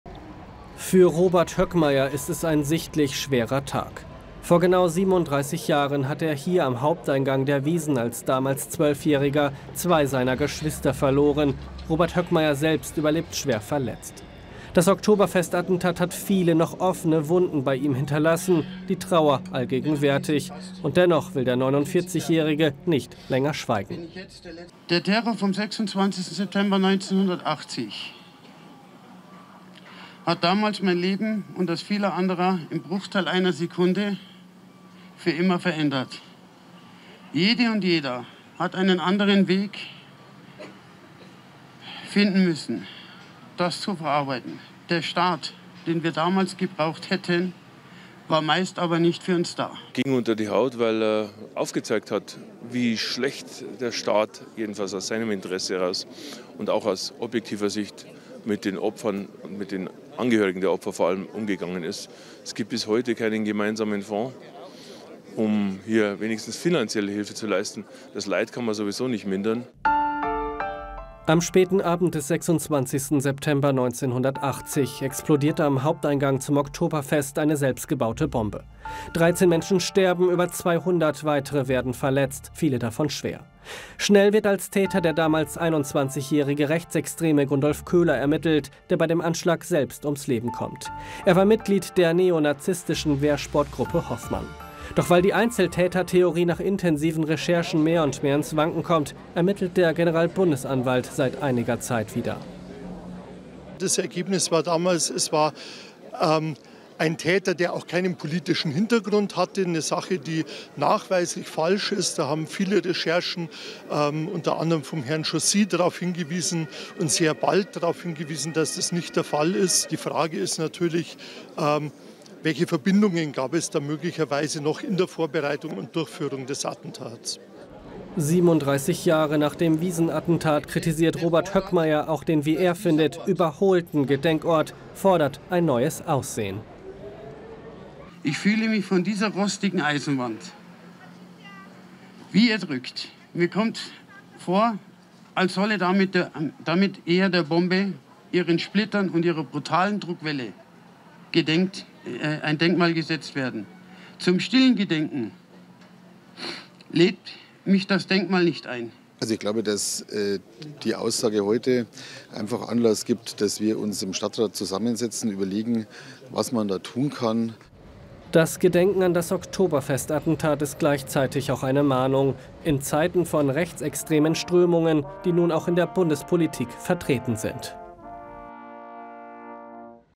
Oktoberfest: Gedenken an Wiesn-Attentat 1980
Heute vor genau 37 Jahren hat das Münchner Oktoberfest den wohl schwärzesten Tag seiner Geschichte erlebt! Am 26. September 1980 explodierte am Haupteingang der Wiesn eine Rohrbombe.